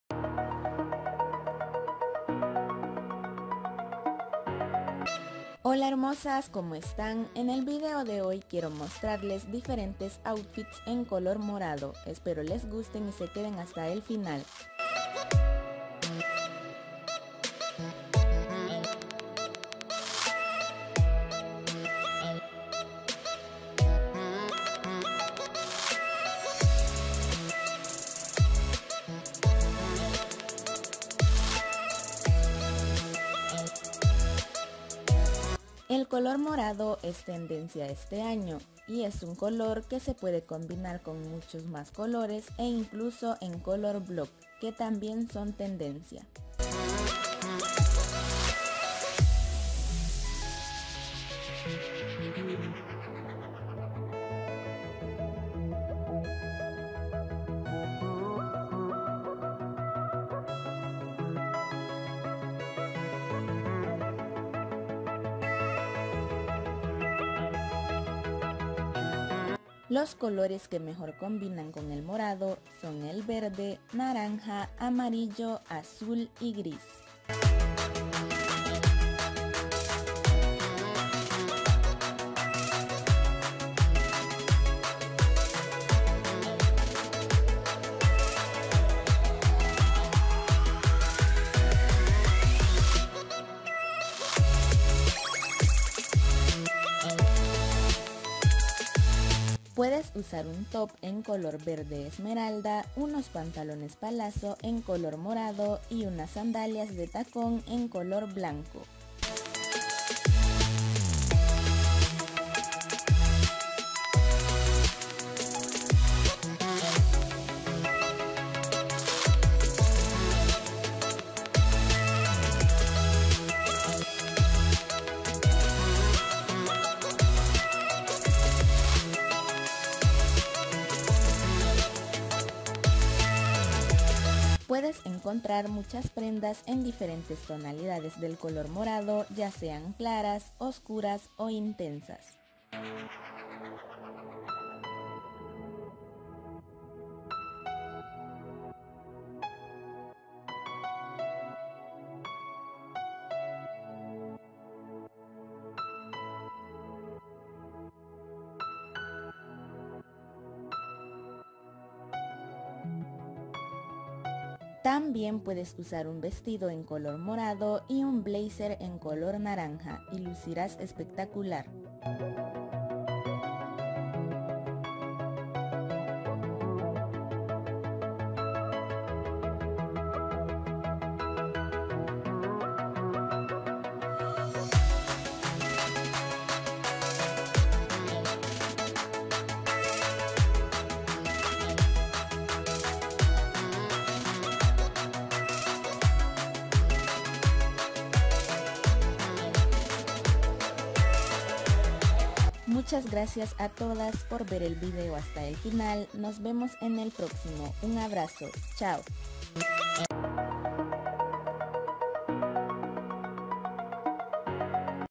IDEAS MODERNAS PARA COMBINAR TUS PRENDAS DE COLOR PÚRPURA DE MODA Y TENDENCIA 2022!!!
Hola hermosas por aquí paso a dejarles estos bellos outfits en color purpura o morado, espero les gusten!

Puedes combinar el color purpura con otros colores que a ti te gusten y consideres que combinan muy bien!

UN ABRAZO Y UN SALUDITO!!!